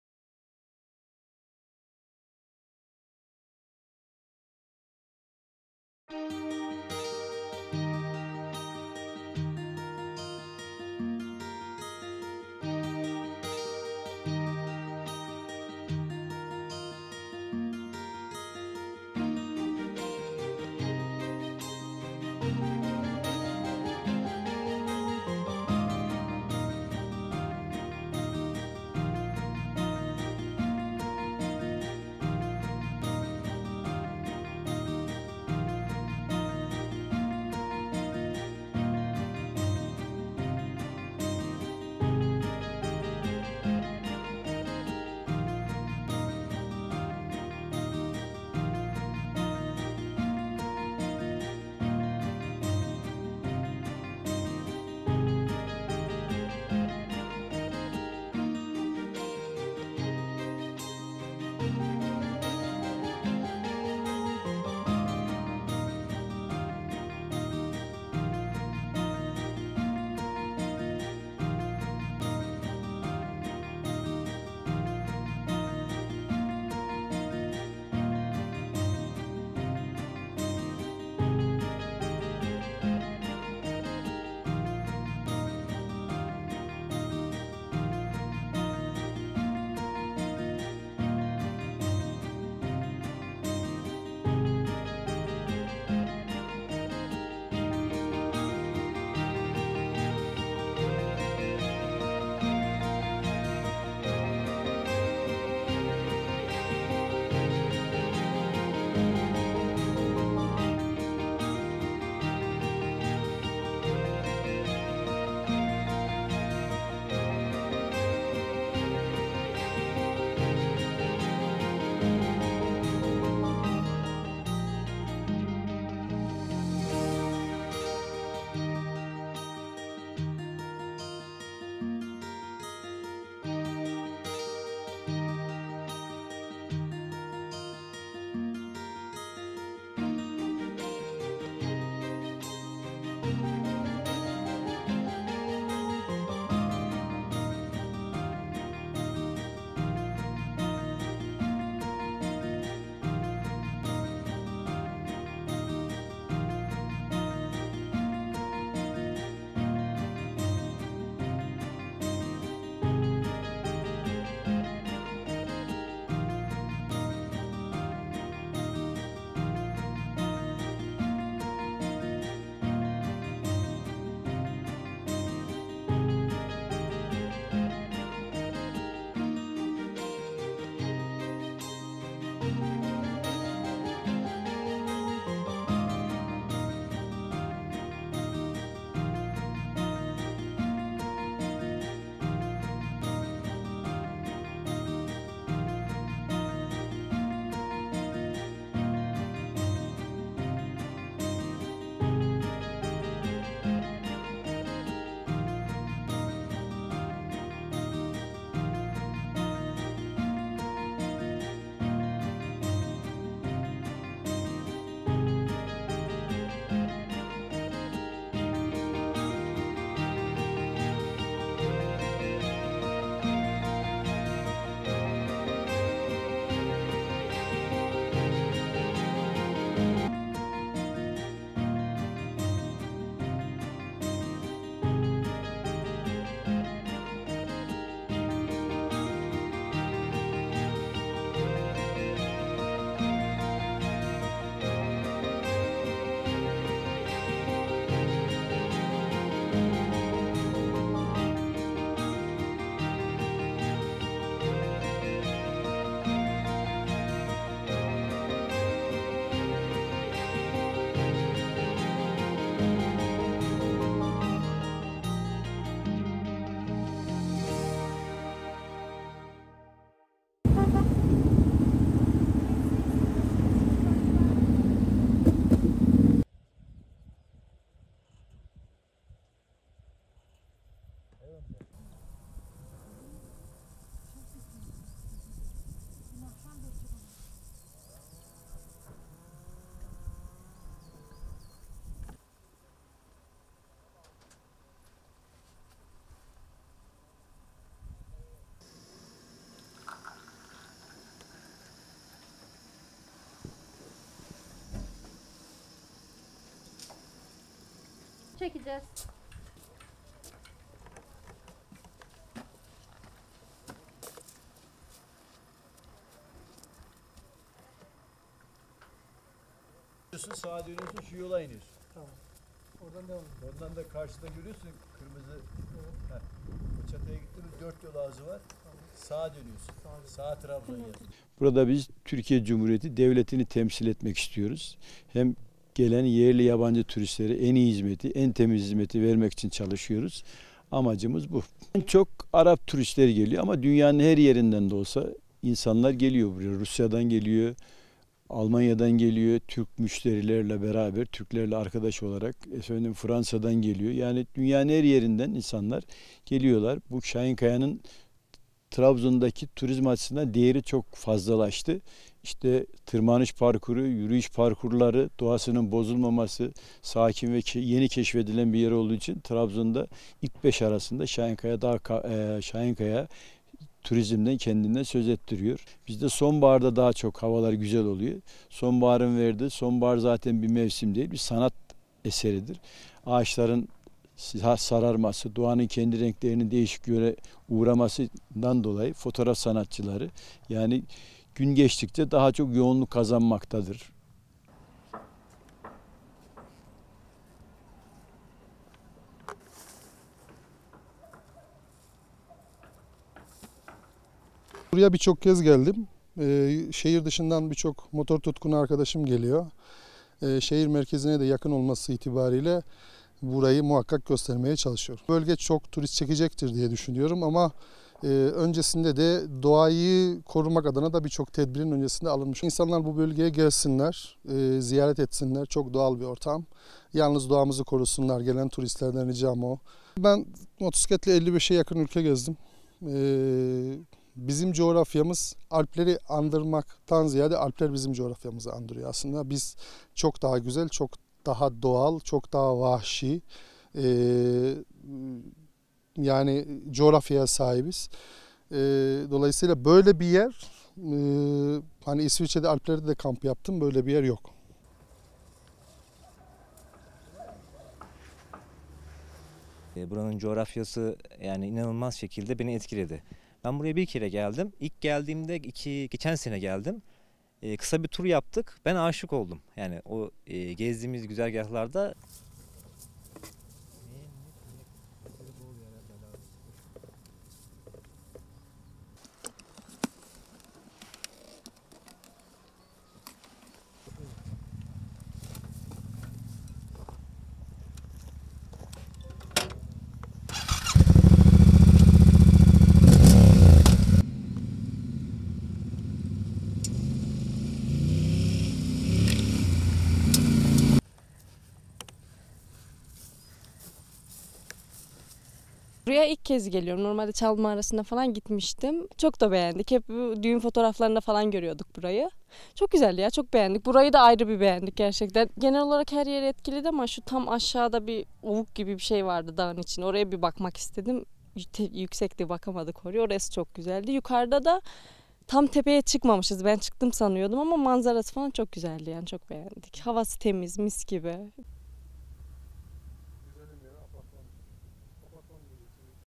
TRABZON - DRON- SONBAHARIN KEYFİNİ YAŞAMAK İSTEYENLER ŞAHİNKAYA'YI TERCİH EDİYOR I TURİZM GÜNLÜĞÜ
TRABZON (AA) - MELTEM YILMAZ KARAKURUM - Trabzon'un Düzköy ilçesindeki Şahinkaya , sonbaharda doğaseverlerin rotasında ilk sıralarda yer alıyor.

 Video: Anadolu Ajansı

Trabzon 'a 65, Düzköy ilçesine ise 11 kilometre mesafedeki Doğankaya Mahallesi'nde yer alan Şahinkaya, uzaktan bakıldığında kaleyi andıran görüntüsü, eteklerindeki çam ormanları ve trekking rotalarıyla ziyaretçilerine ev sahipliği yapıyor.

İsviçreli tırmanıcı Michel Piola başta olmak üzere Türkiye'nin önemli kaya tırmanıcılarının açtığı yeni rotalarla da adından söz ettiren Şahinkaya, özellikle kaya tırmanışı yapmak isteyenleri kendisine çekiyor.
Türkiye'nin yanı sıra yurt dışından çok sayıda dağcıya ev sahipliği yapan, kaya tırmanışı dışında motosiklet, kamp ve karavan turizmine de müsait olan Şahinkaya, dağ yürüyüşü ile foto safariye elverişli alanlarıyla turizm çeşitliliği sağlıyor.

Özellikle kuş bakışı manzarasıyla sosyal medyada öne çıkan Şahinkaya'ya gelen ziyaretçiler, otantik dağ evlerinde konaklama, yöresel lezzetleri tatma, yol güzergahındaki Çal Mağarası'nı ziyaret etme ve atla gezinti imkanı bulabiliyor.

Yerli turistlerin yanı sıra Avrupa ve Körfez ülkelerden gelen turistlerden rağbet gören Şahinkaya, doğada renk değişiminin yaşandığı bugünlerde adından daha fazla söz ettiriyor.

- "Turistlere en iyi ve temiz hizmeti vermek için çalışıyoruz"

Şahinkaya'da işletme sahibi Zühtü Bulut, AA muhabirine, öğretmenlikten emekli olduktan sonra çocukluğunun ve gençliğinin geçtiği bölgede turizme hizmet etmek için kolları sıvadığını söyledi.

Bu kapsamda bir işletme kurduğunu dile getiren Bulut, "Burada Türkiye Cumhuriyeti Devleti'ni temsil etmek istiyoruz. Gelen yerli ve yabancı turistlere en iyi ve temiz hizmeti vermek için çalışıyoruz, amacımız bu." dedi.
Bulut, en çok Körfez ülkelerden turistler ağırladıklarını ifade ederek, sözlerini şöyle sürdürdü:

"En çok Araplar olmak üzere Rusya'dan, Almanya'dan, Fransa'dan yani dünyanın her yerinden insanlar geliyor. Şahinkaya'nın turizm açısından değeri çok fazlalaştı. Tırmanış ve yürüyüş parkurları, doğasının bozulmaması, sakin ve yeni keşfedilen bir yer olduğu için Trabzon'da ilk 5 arasında Şahinkaya turizmde kendinden söz ettiriyor."

Şahinkaya'nın sonbaharda daha fazla ilgi gördüğünü belirten Bulut, "Sonbahar zaten bir mevsim değil, bir sanat eseridir. Ağaçların sararması, doğanın kendi renklerinin değişikliğe uğramasından dolayı gün geçtikçe de doğasever ve fotoğraf sanatçılarından yoğun ilgi görüyor." diye konuştu.

- "İsviçre'de Alpler'de de kamp yaptım ama böyle bir yer yok"

Kendi gibi motosiklet tutkunu arkadaşıyla Şahinkaya'ya gelen Ali Bekir Özşahin de Şahinkaya'ya birçok kez geldiğini anlatarak, "Şehir dışından birçok motor tutkunu arkadaşım geliyor. Şehir merkezine de yakın olması nedeniyle burayı muhakkak göstermeye çalışıyorum." ifadelerini kullandı.
Şahinkaya'nın turizmde kesinlikle ön plana çıkması gereken bir yer olduğuna dikkati çeken Özşahin, "Bu bölge çok turist çekecektir diye düşünüyorum ama öncesinde de doğayı korumak adına birçok tedbirin alınmış olması lazım. Turistler bu bölgeye gelsinler, ziyaret etsinler. Çok doğal bir ortam ama yalnız doğamızı da korusunlar." şeklinde konuştu.
Özşahin, motosikletiyle 55'e yakın ülke gezdiğini belirterek, "Bizim coğrafyamız Alpler'i andırmaktan ziyade, aslında Alpler bizim coğrafyamızı andırıyor. Biz çok daha güzel, çok daha doğal, çok daha vahşi coğrafyaya sahibiz. İsviçre'de Alpler'de de kamp yaptım ama böyle bir yer yok." diye konuştu.

İstanbul'dan motosikletle bölgeye gelen Fuat Altan Bedişkan da coğrafyanın kendisini inanılmaz şekilde etkilediğini vurgulayarak, "İlk geçen sene Trabzon'a geldim, kısa bir tur yaptık. Buraya aşık oldum." dedi.

Akrabalarıyla ilk defa geldiği Şahinkaya'nın doğasından ve manzarasından çok etkilendiklerini anlatan Merve Çebi de "Sosyal medyada burayı hep düğün fotoğraflarında falan görüyorduk. Çok güzel, çok beğendik." diye konuştu.

Çebi, orman içindeki parkurda gezerek Şahinkaya'ya çıktıklarını ifade ederek, sözlerini şöyle tamamladı:

"Genel olarak her yeri beni etkiledi ama tam aşağıda dağın içinde oyuk gibi bir şey vardı, oraya bakmak istedim. Yüksekti bakamadık oraya, orası çok güzeldi. Yukarıda da tam tepeye çıkmamışız, ben çıktım sanıyordum ama manzarası çok güzeldi, çok beğendik. Havası temiz, mis gibi."